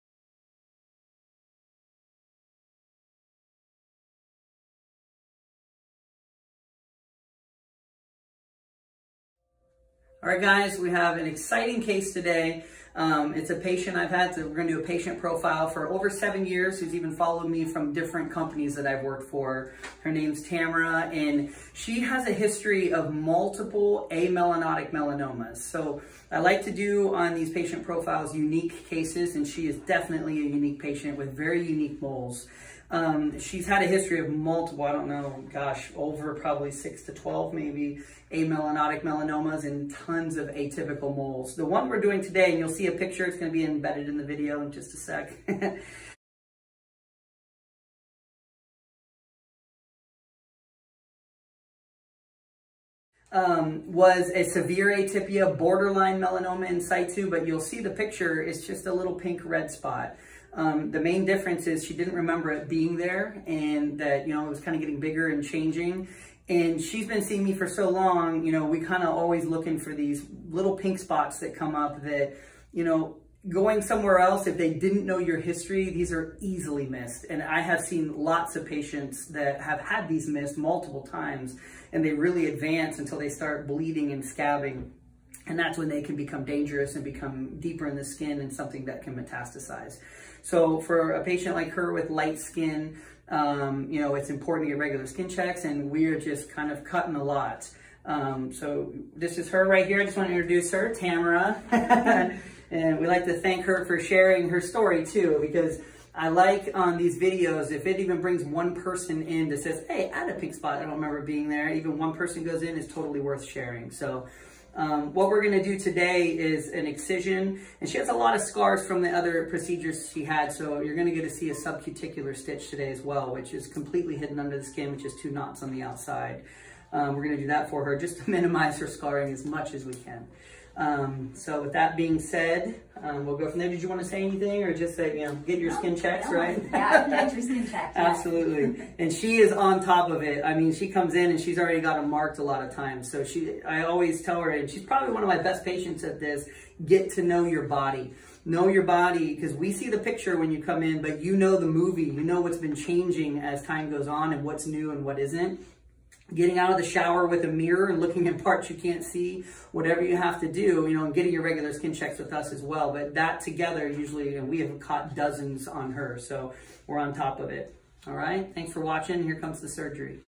AMELANOTIC MELANOMA-IN-SITU. Patient profiles-Tamara. Part 1 of 3. Unique dangerous mole. MrPopZit
Amelanotic (means no melanin-so it’s not dark) Melanoma can be very dangerous because it’s commonly not biopsied due to not looking suspicious for atypical cells. This mole was just turning into Melanoma (“in situ”) so we caught it early before it could have been deadly. This surgery completely cleared the mole so nothing further was needed. It’s a long surgery so I split it into 3 parts. All 3 videos will be released in the next 12 hours - Intro, surgery, and closure. The closure will showcase the running sub cuticular stitch which leaves a less noticeable scar. Thank you to my patient Tamara for sharing her story! Dermatology procedure, pathology discussion, wide excision, and closure. MrPopZit.